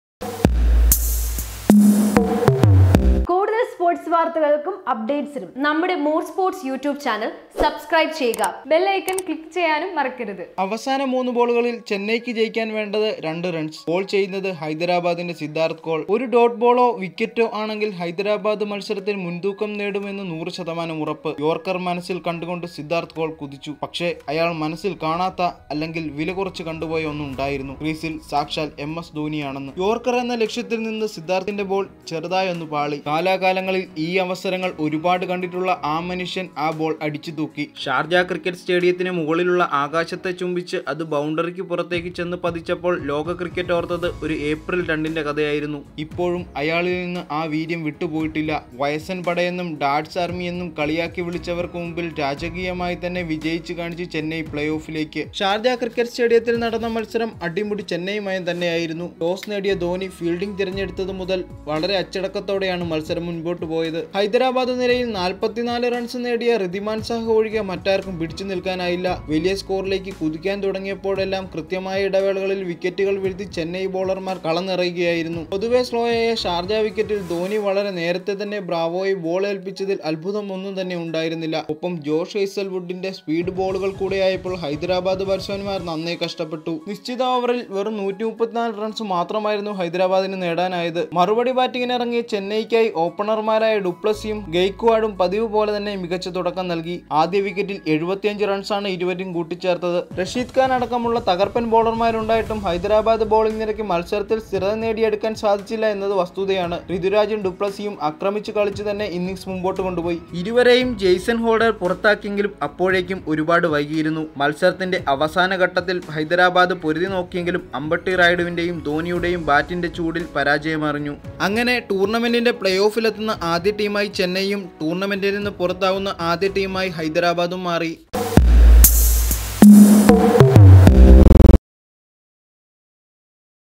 വീണ്ടും ധോണി സ്റ്റൈല്‍ ഫിനിഷ് : മഹിയും പിള്ളാരും പ്ലേയോഫില്‍....
അവസാന മൂന്ന് ബോളുകളിൽ ചെന്നൈയ്ക്ക് ജയിക്കാൻ വേണ്ടത് രണ്ടു റൺസ്... ബോൾ ചെയ്യുന്നത് ഹൈദരാബാദിന്റെ സിദ്ധാർത്ഥ് കോള്‍... ഒരു ഡോട്ട് ബോളോ വിക്കറ്റോ ആണെങ്കിൽ ഹൈദരാബാദ് മത്സരത്തിൽ മുൻതൂക്കം നേടും എന്ന് നൂറു ശതമാനം ഉറപ്പ്... യോര്‍ക്കര്‍ മനസ്സില്‍ കണ്ട് സിദ്ധാർത്ഥ് കോൾ കുതിച്ചു.. എന്നാൽ അയാൾ മനസ്സിൽ കാണാത്ത, അല്ലെങ്കിൽ വിലകുറച്ചു കണ്ടുപോയ ഒന്നുണ്ടായിരുന്നു... ക്രീസില്‍ സാക്ഷാല്‍ എംഎസ് ധോണിയാണെന്ന്.

for Advertisement WhatsApp to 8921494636
ക്രിക്കറ്റ് മലയാളം , ക്രിക്കറ്റ് , Cricket , ക്രിക്കറ്റ് വാർത്തകൾ  , moresports
Sporta Malayalam